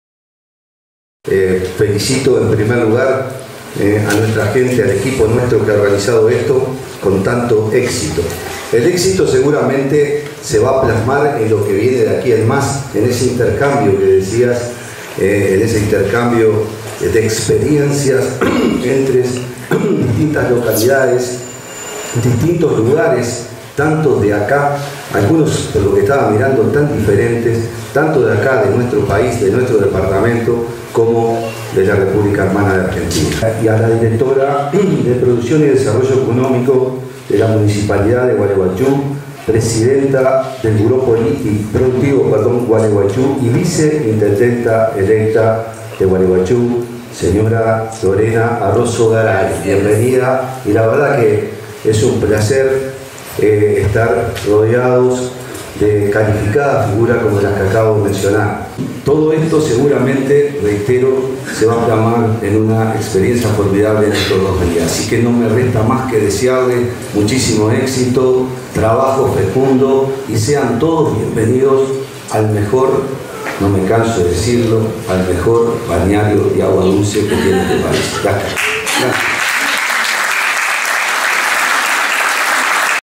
Tercer Encuentro Binacional
El 3er Encuentro Binacional fomenta la integración de los dos pueblos hermanos.

Tel.: 1935 internos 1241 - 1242